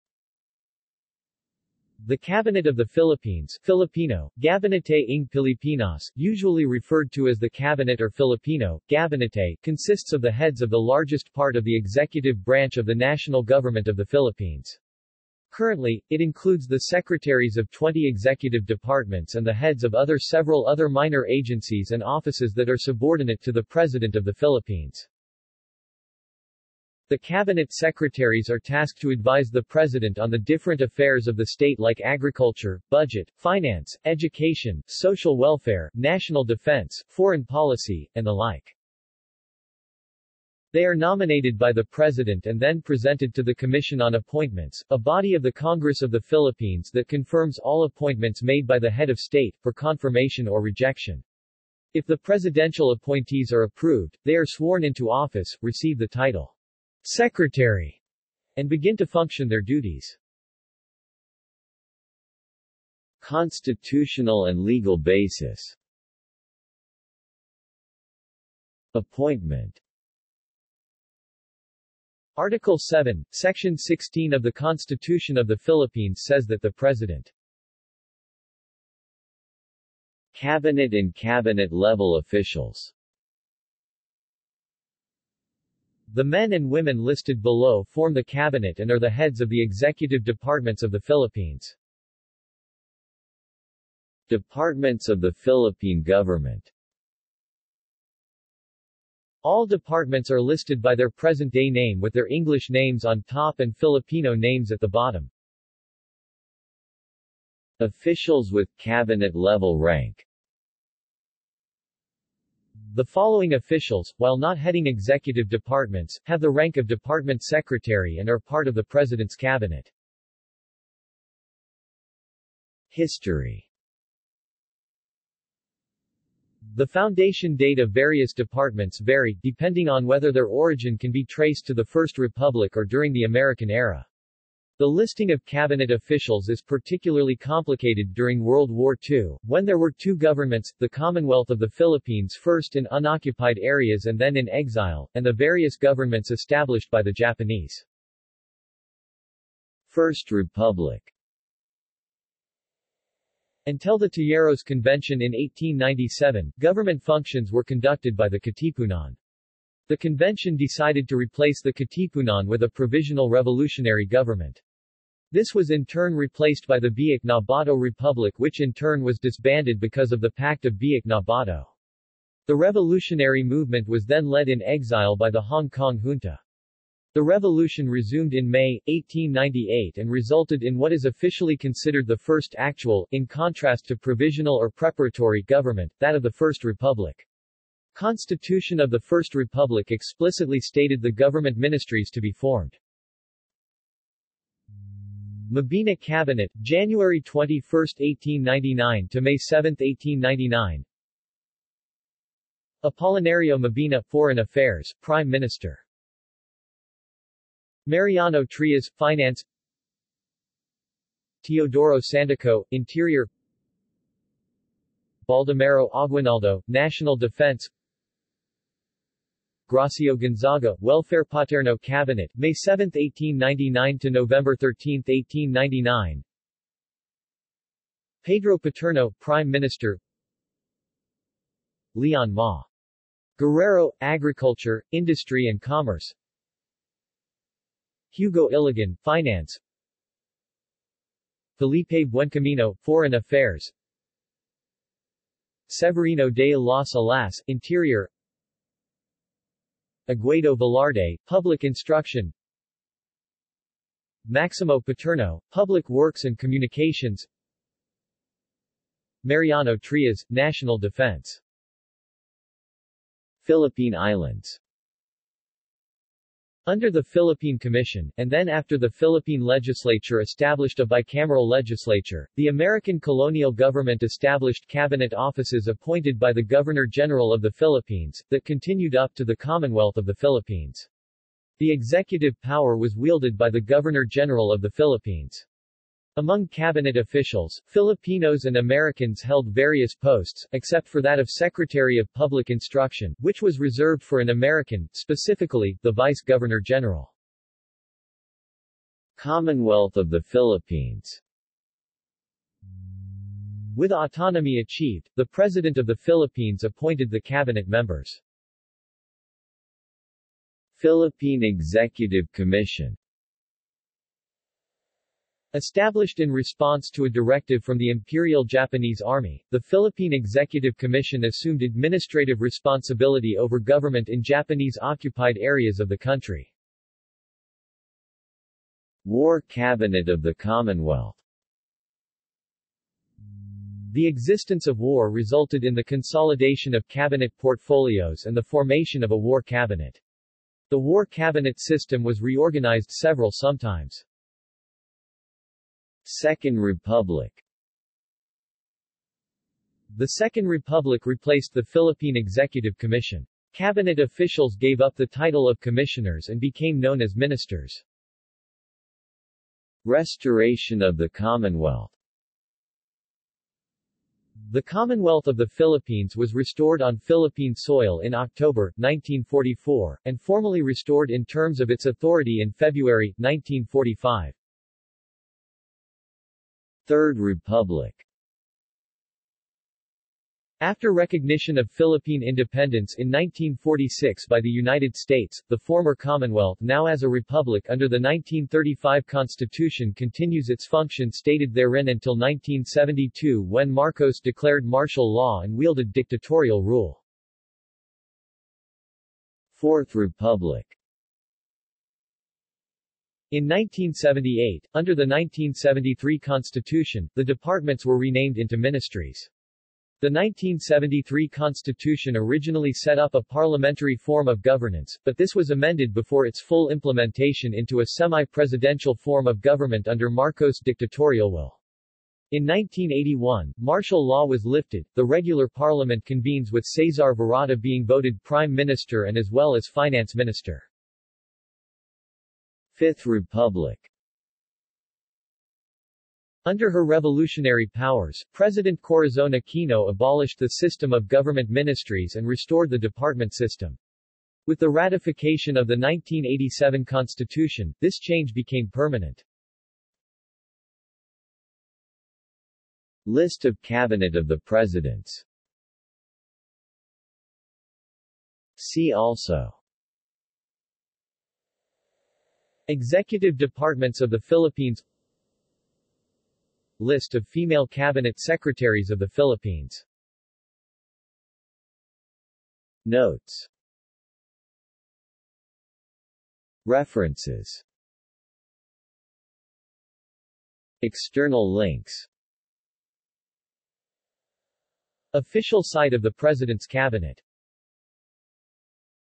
Cabinet of the Philippines | Wikipedia audio article
This is an audio version of the Wikipedia Article:
Cabinet of the Philippines

SUMMARY
The Cabinet of the Philippines (Filipino: Gabinete ng Pilipinas, usually referred to as the Cabinet or Filipino: Gabinete) consists of the heads of the largest part of the executive branch of the national government of the Philippines. Currently, it includes the secretaries of 20 executive departments and the heads of other several other minor agencies and offices that are subordinate to the President of the Philippines.
The Cabinet secretaries are tasked to advise the President on the different affairs of the state like agriculture, budget, finance, education, social welfare, national defence, foreign policy, and the like.
They are nominated by the President and then presented to the Commission on Appointments, a body of the Congress of the Philippines that confirms all appointments made by the head of state, for confirmation or rejection. If the presidential appointees are approved, they are sworn into office, receive the title "Secretary," and begin to function their duties.